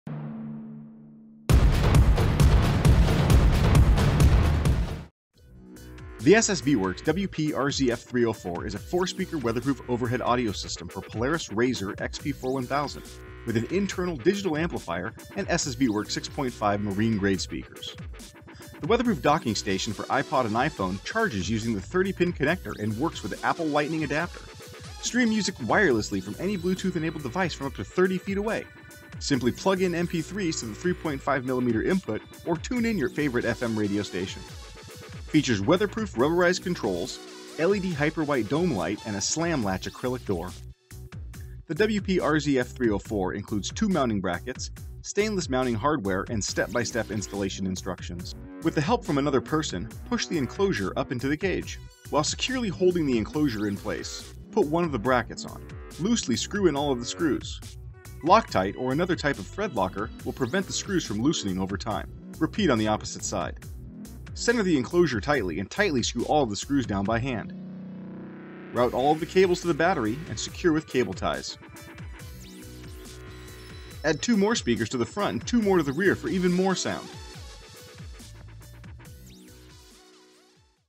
SSV Works WP RZF304 Polaris RZR xp4 1000 Overhead Speaker System
The Best Just Got Better AND Louder SSV Works new iPod Weather Proof audio system for the Polaris RZR XP4 1000 provides Hi-Def stereo sound. The WP-RZ3O4 clamps in overhead to your stock cage, installs in 15 minutes with a simple 2 wire hook up and is totally weather proof. Like all SSV Works systems, the enclosure is constructed of fiberglass for maximum durability and the best sound quality. All systems include wiring, mounting hardware, and step-by-step instructions. Features: - Weatherproof Docking station for iPod / iPhone, no radios needed - Water tight locker with slam-latch polycarbonate door - Charges iPod, iPhone and touch - internal 50 watt x4 ultra-efficient amp for UTV's charging system - Waterproof marine 2-way 6.5" speakers - 4 LED hyperwhite dome light - On board bezel mounted rubberized controls - iPod control (Volume, Track FWD/BCK, Play/Pause) - 3.5mm AUX input for MP3 or Sat Radio - FM tuner with backlit display and external antenna - Optional RCA low-level output for system expansion (requires WP-HRCA adapter).